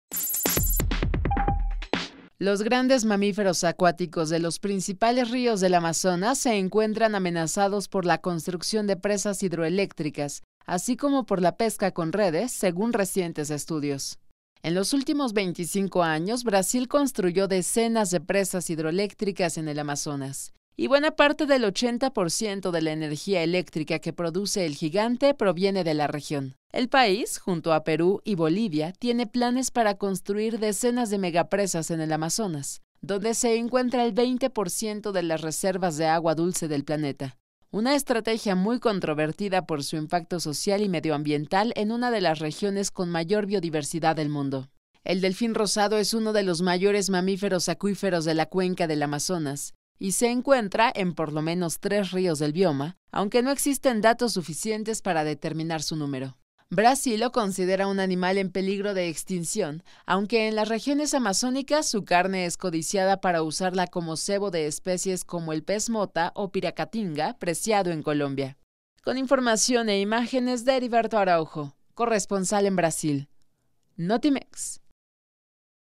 Desarrollo amenaza al mítico delfín rosado del Amazonas
Mamíferos acuáticos de los principales ríos del Amazonas se encuentran amenazados por la construcción de presas hidroeléctricas, así como por la pesca con redes